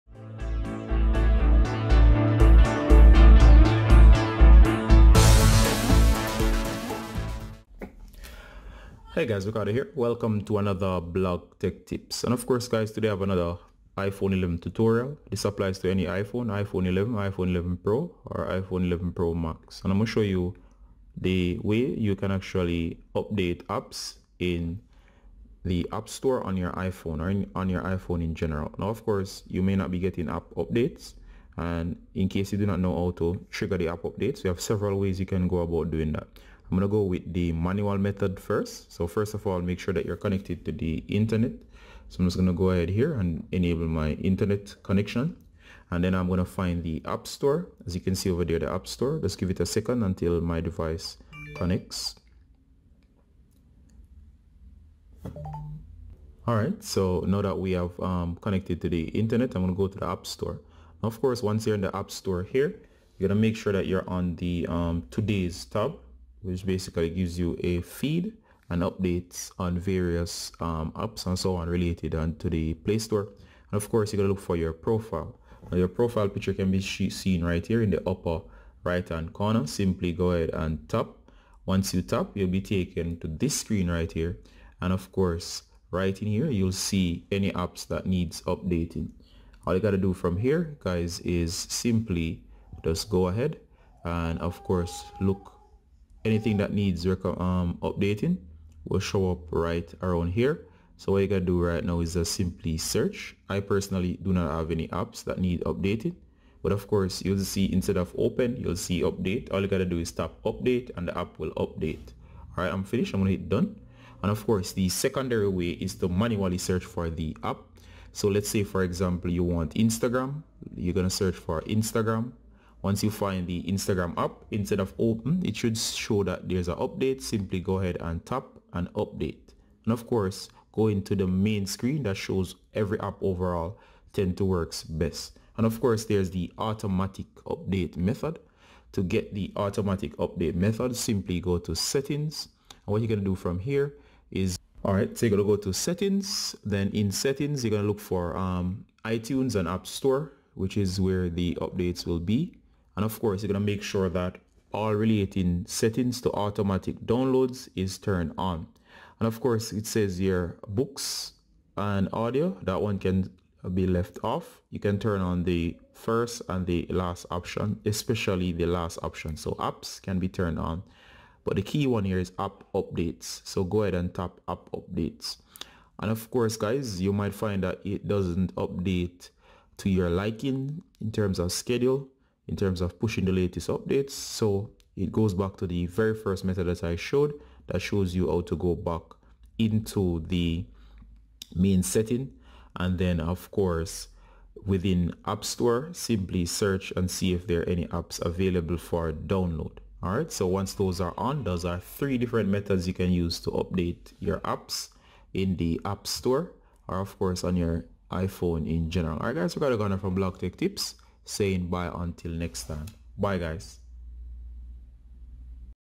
How to Update Apps on iOS 13
Learn three (3) ways on How to Update Apps on iOS 13 with your iPhone. This includes any iPhone 11 or older and newer devices. If you want to manually update your apps or set them to auto update learn how to do so and more.